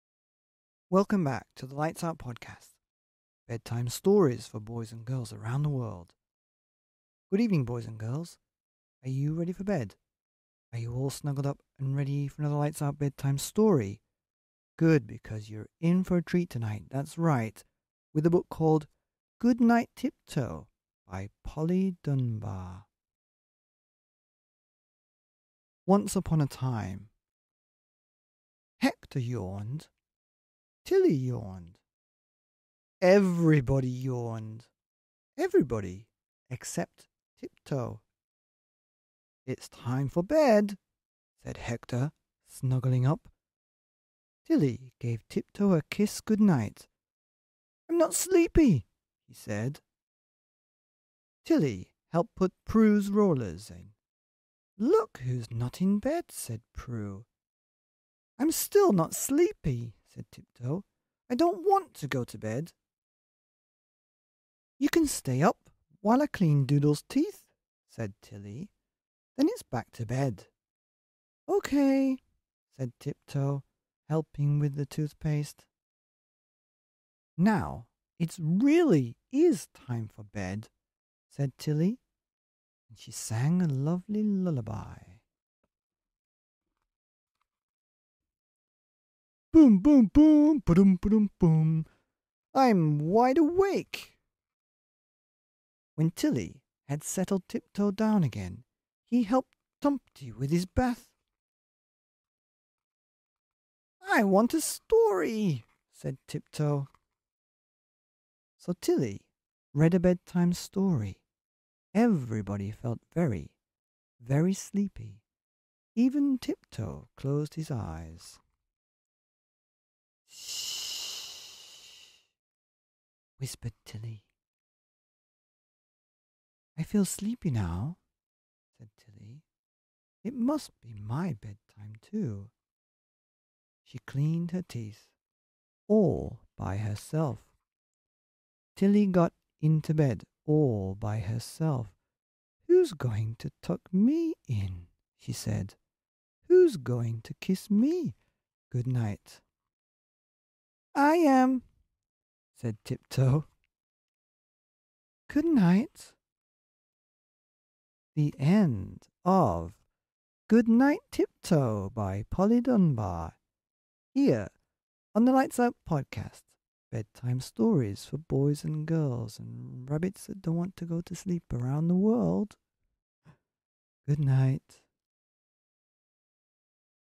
Goodnight Tiptoe by Polly Dunbar - Read by Martyn Kenneth
On The Lights Out Podcast. Now with over 500 Ad-free Bedtime Stories for Boys and Girls around the world - FOREVER